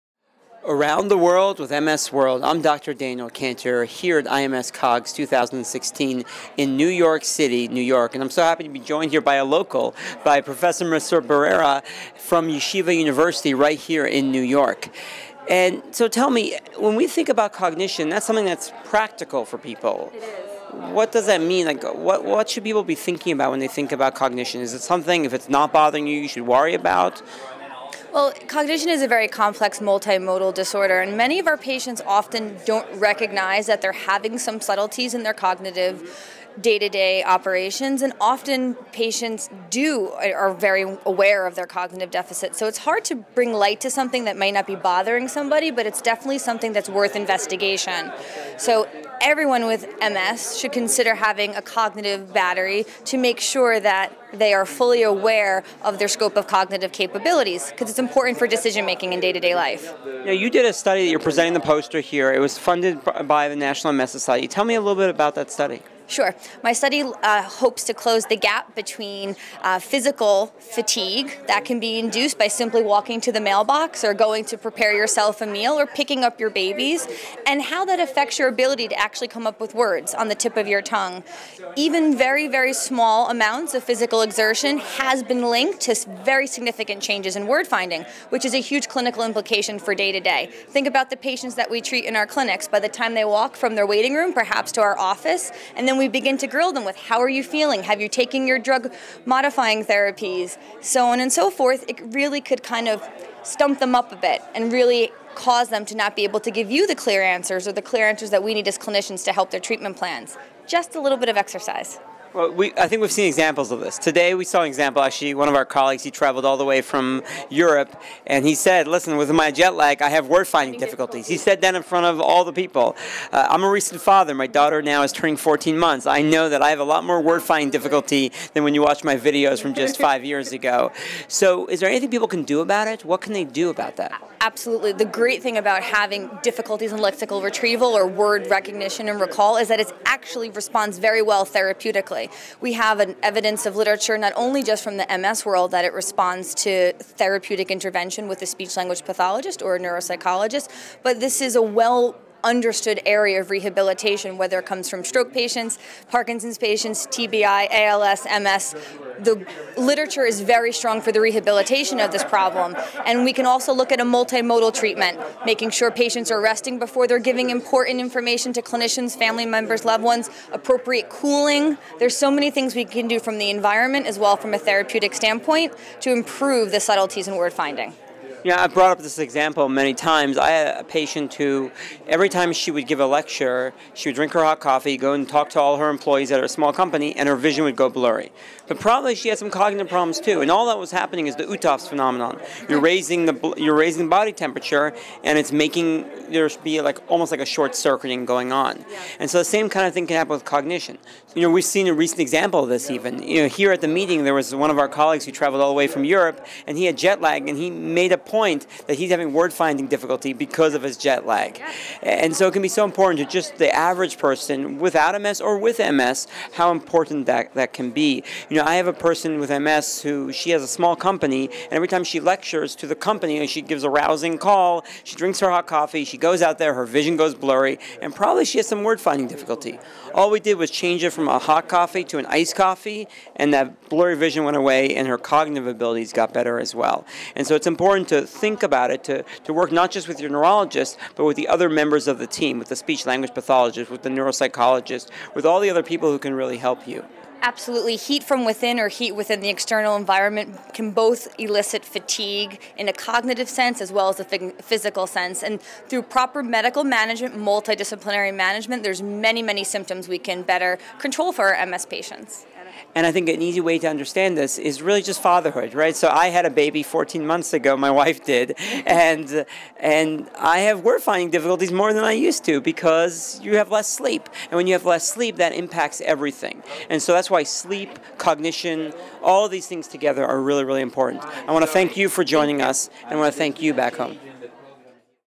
Dr. Kantor interviews Marissa Barrera, MS, MSCS, CCC-SLP, about the practical aspects of cognition
Dr. Kantor interviews Marissa Barrera, MS, MSCS, CCC-SLP, Aspire Center for Health, NY, about the practical aspects of cognition, including having a cognitive battery to make sure they are aware of their scope of cognitive capabilities.